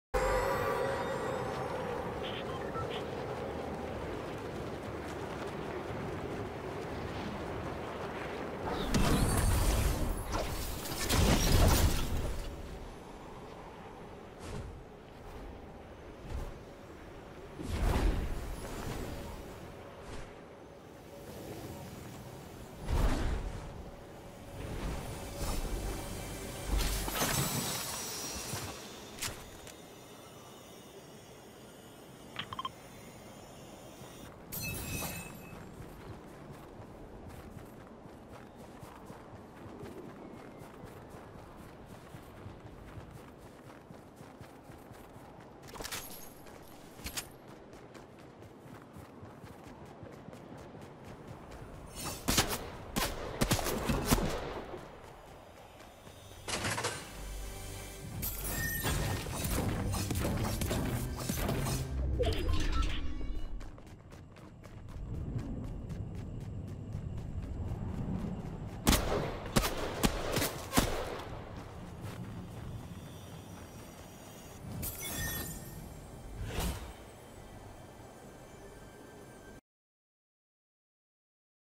Collect Omni Chips at Logjam Lumberyard!!!
What's up guys, In today's video i'm going to show you How To Complete OMNI SWORD QUEST.